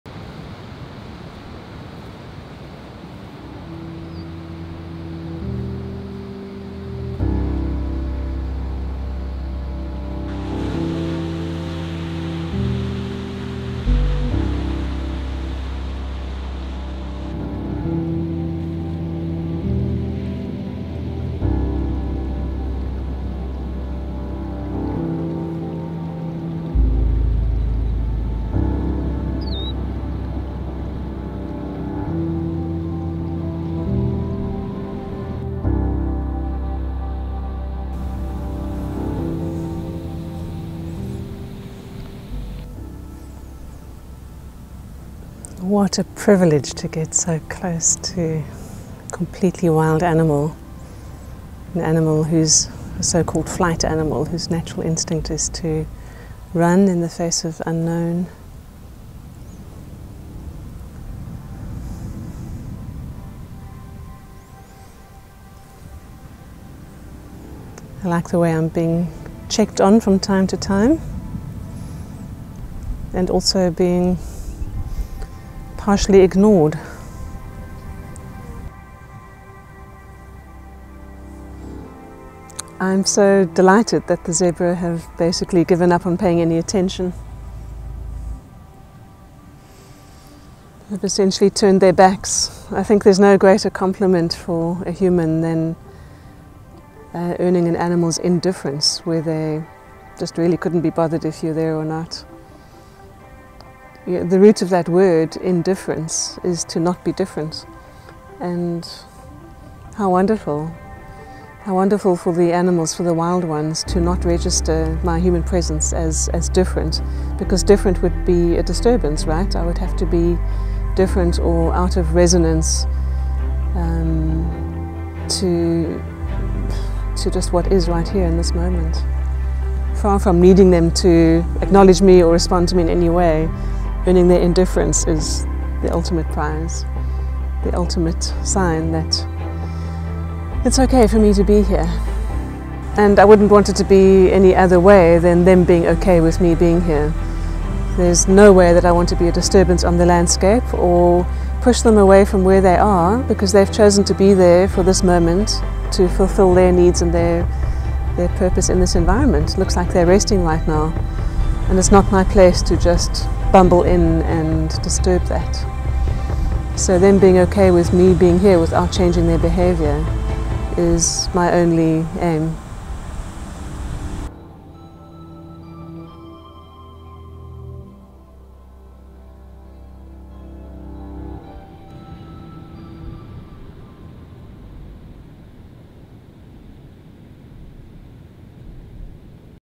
Animal Indifference is a compliment
During a spontaneous walk in Cape Point National Park in South Africa, Anna comes across two zebra. Staying a respectful distance away, she comments on the privilege of earning animals’ indifference and being able to co-exist in a shared space for a sweet moment of peaceful connection.